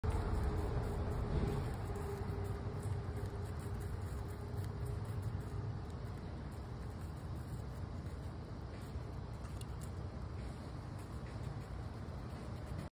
Squirrel peels apple w teeth! 😂🥰
Squirrel peeling an apple! Too cute animal video! Funny video!